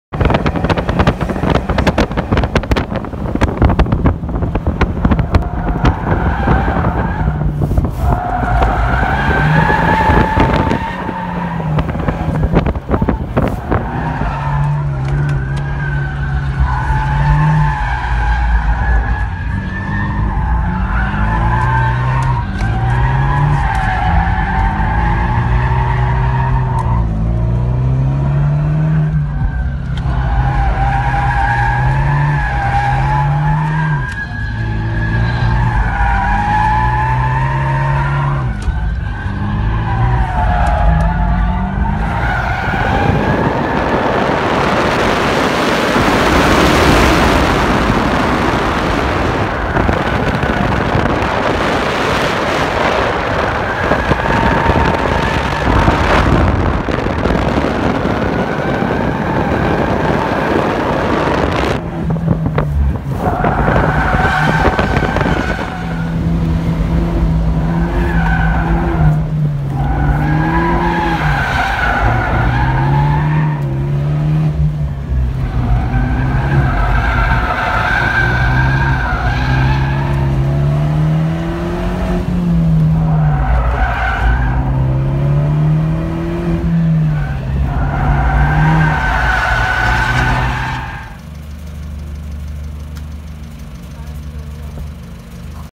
Abarth 124 drift
Some fun with Abarth 124 in Cerrina Race Track